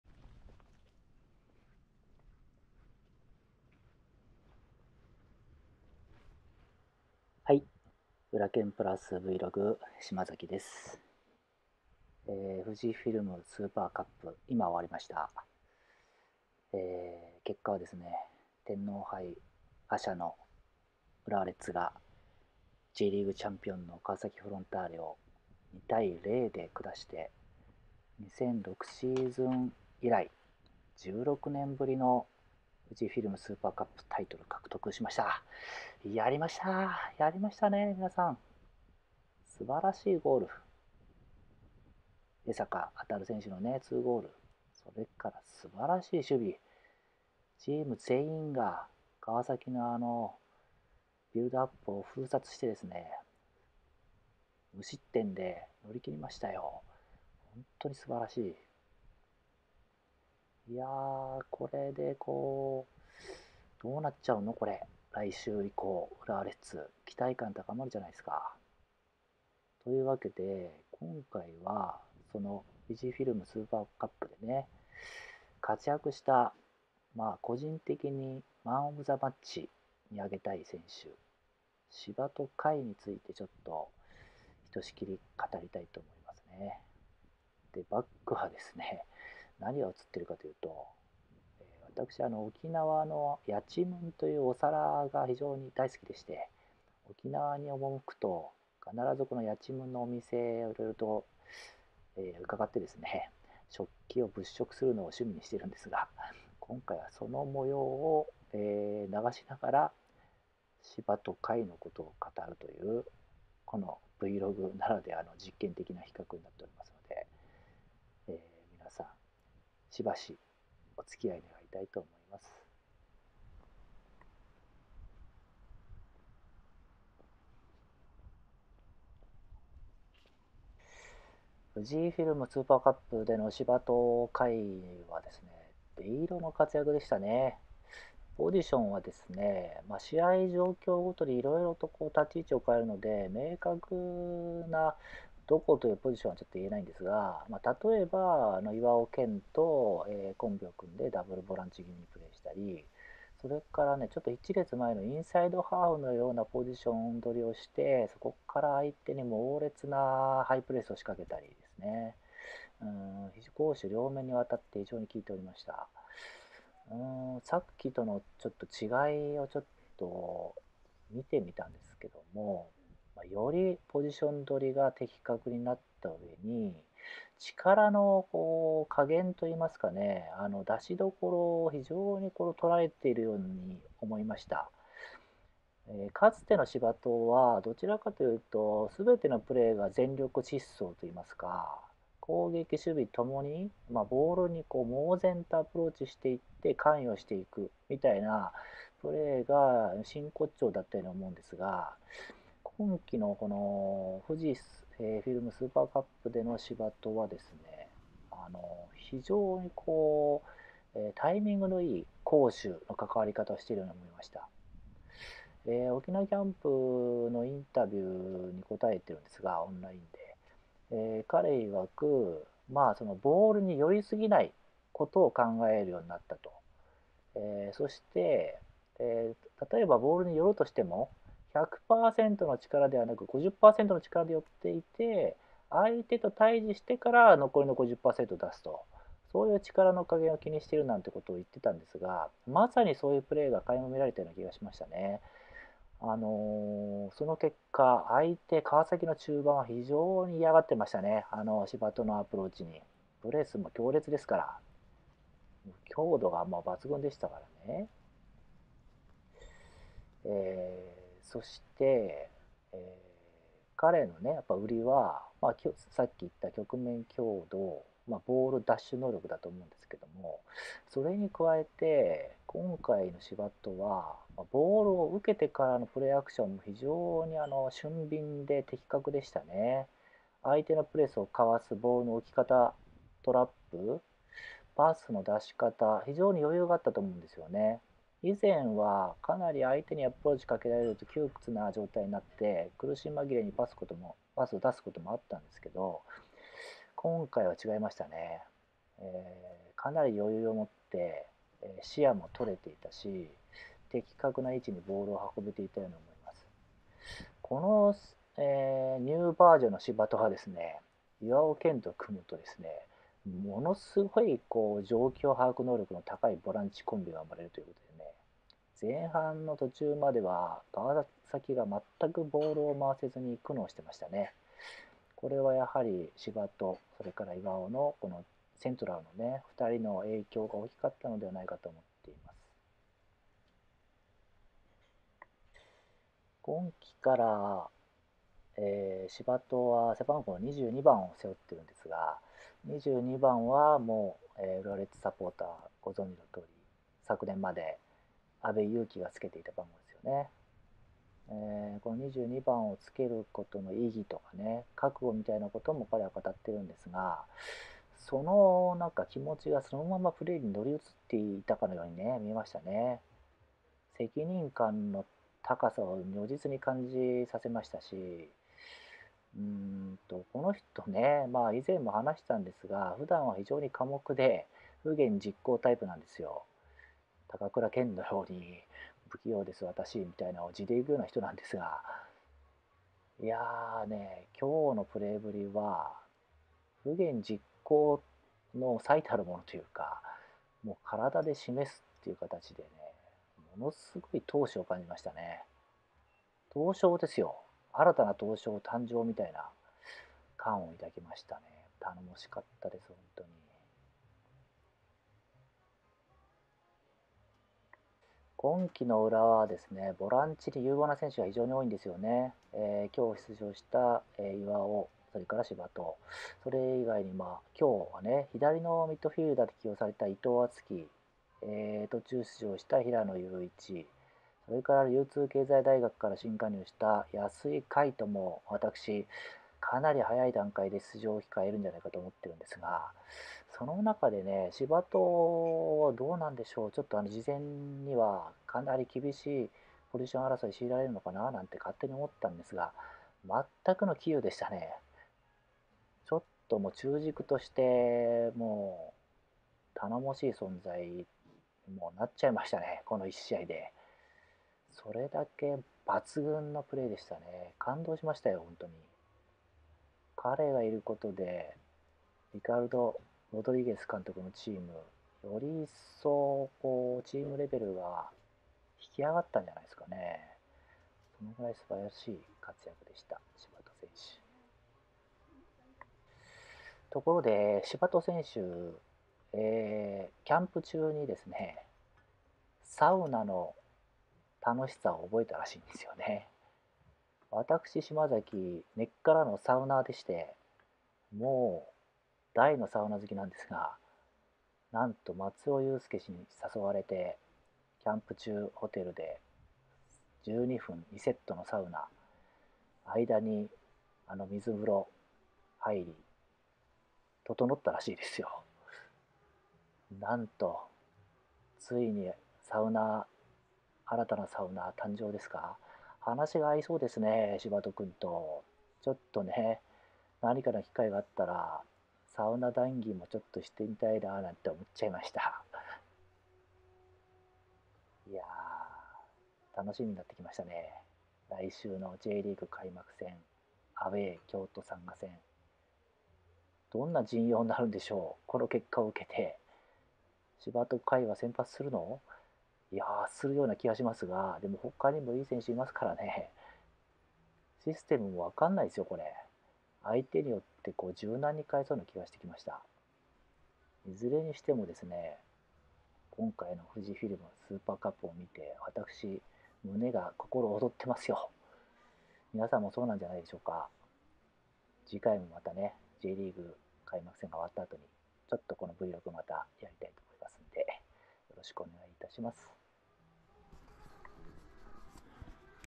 浦和レッズ／『FUJIFILM SUPER CUP 2022』の浦研プラス選定MOMは柴戸海！【浦研プラスVlog#008】
Vlog始めました。
スポーツライターの島崎英純が、日々の徒然をお伝えするユルユル動画です。

『FUJIFILM SUPER CUP 2022』川崎フロンターレ0-2浦和レッズ
浦和レッズ、2006シーズン以来16年ぶりのスーパーカップ制覇！

※『FUJIFILM SUPER CUP 2022』川崎フロンターレvs浦和レッズの試合レビュー原稿は明日、2月12日（日）朝に掲載予定！

やちむんの詳細については、沖縄観光情報WEBサイトのこちらをご参照ください。